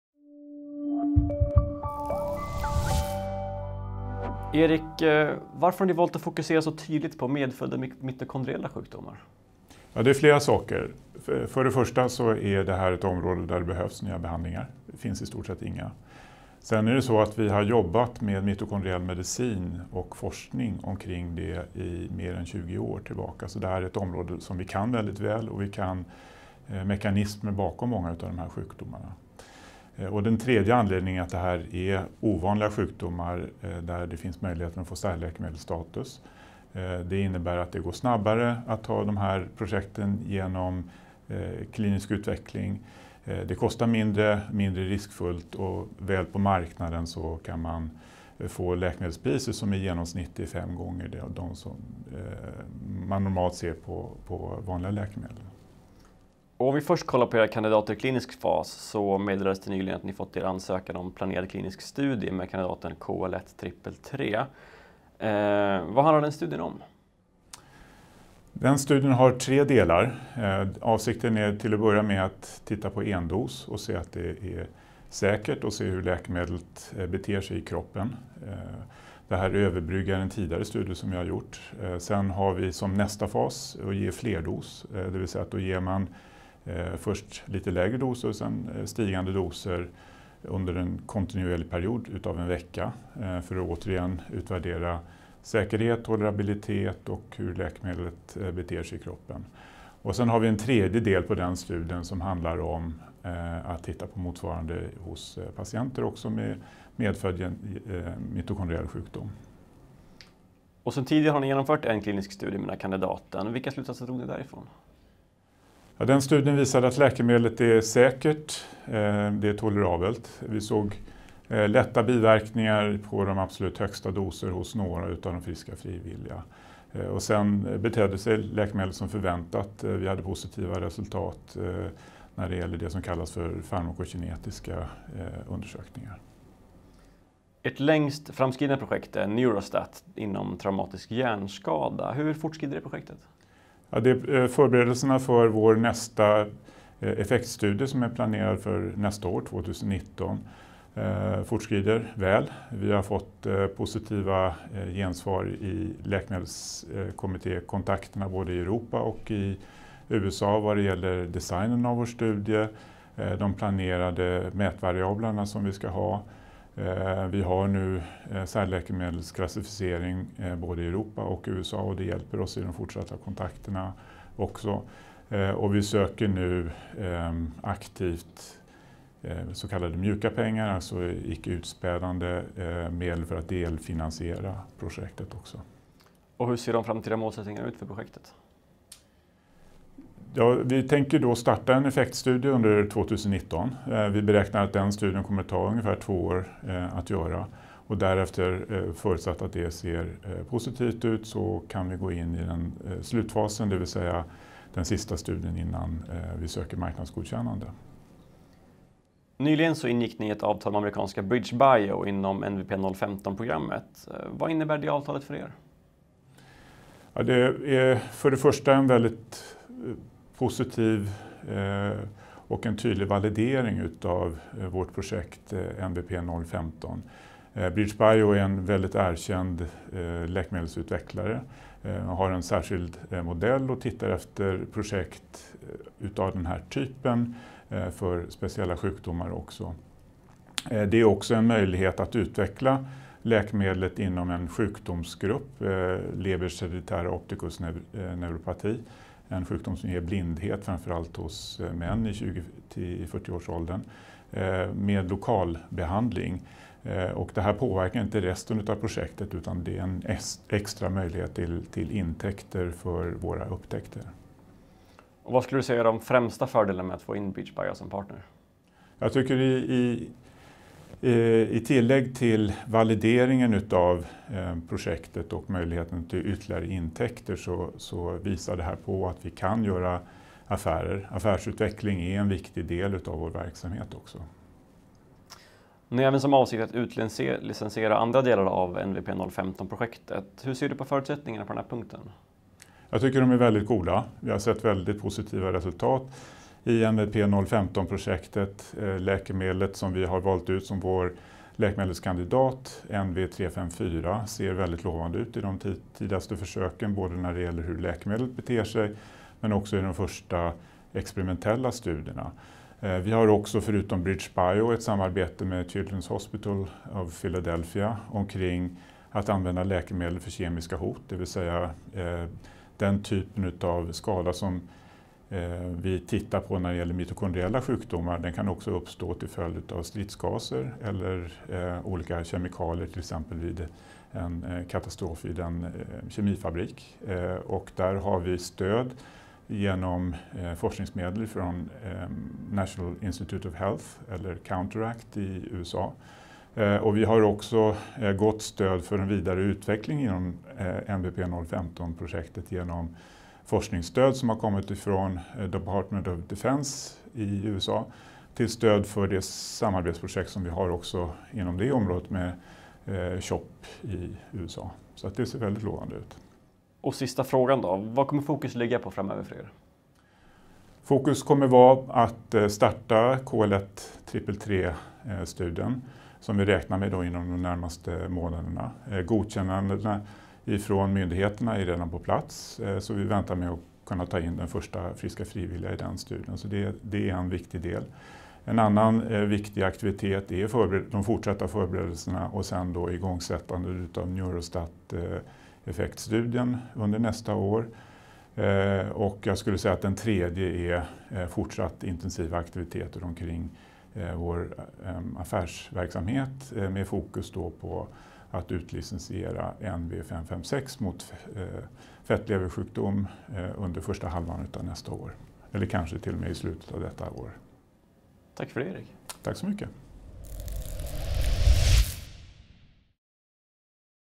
NeuroVive Pharmaceutical
NeuroVive Pharmaceutical utvecklar läkemedel för sällsynta mitokondriella sjukdomar, ett område som vd Erik Kinnman anser att bolaget har väldigt lång erfarenhet av.

I en intervju med Finwire.tv berättar Erik om läget för de olika forskningsprojekten och om vad bolaget fokuserar på just nu.